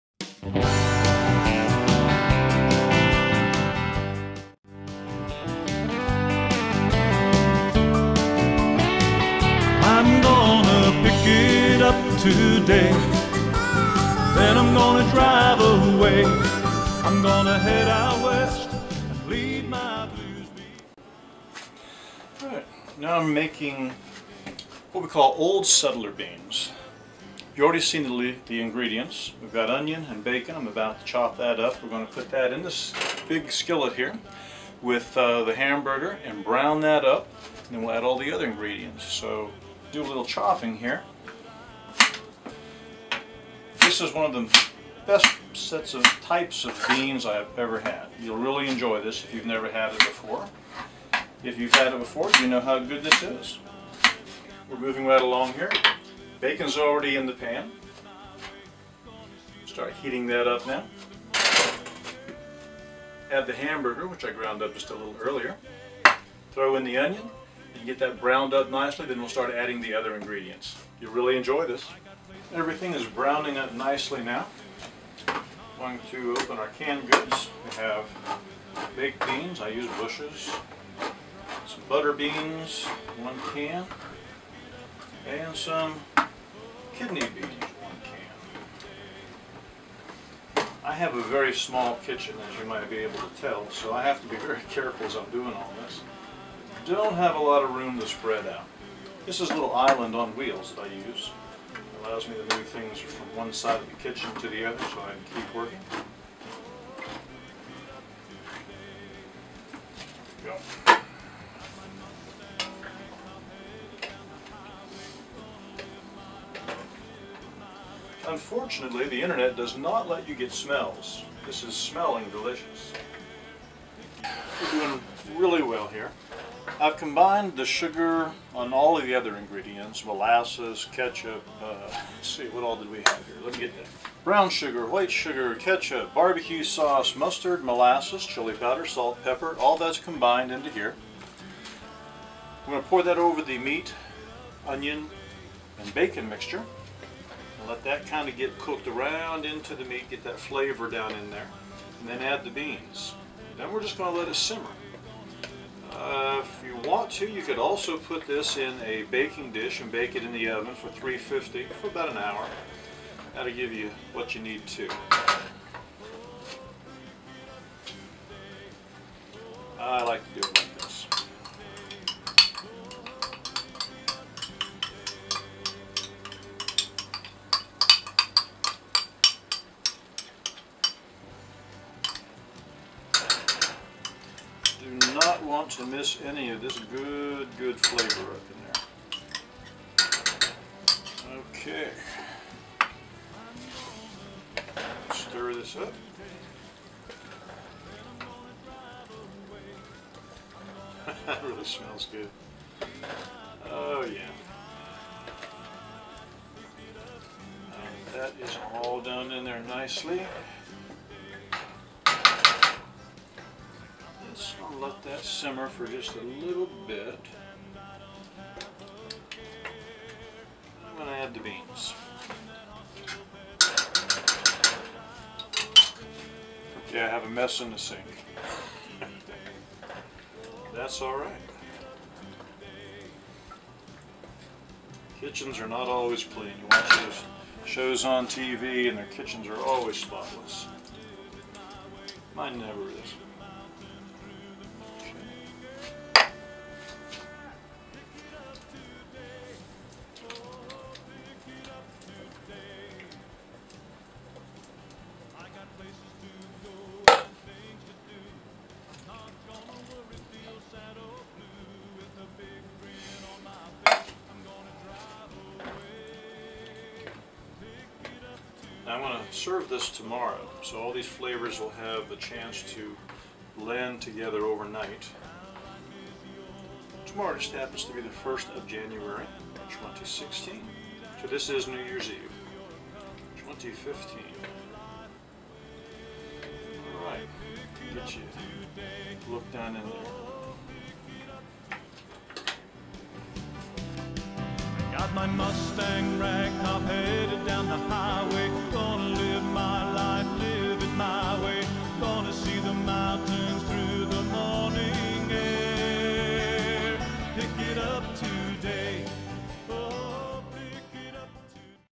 Cooking With Gary, "Ole Settler Beans" (Episode 2)
½ Lb Ground Beef
½ Lb Bacon, Chopped
1 Onion Chopped

½ Cup Brown Sugar
½ Cup White Sugar
¼ Cup Catsup
¼ Cup Barbecue Sauce
2 Tablespoon Mustard
2 Tablespoon Molasses
1 Teaspoon Chili Powder
½ Teaspoon Salt
¼ Teaspoon Pepper

1 lb Can Kidney Beans
1 28 Oz can Pork and Beans Drained
1 Lb Can Butter beans drained

Brown the first 3 Items.

Combine the remaining ingredients, except for the beans and pour over browned meat.  Mix well and add the beans.  Pour into a large baking dish and bake at 350 for 1 hour or cook in deep heavy skillet on stove top.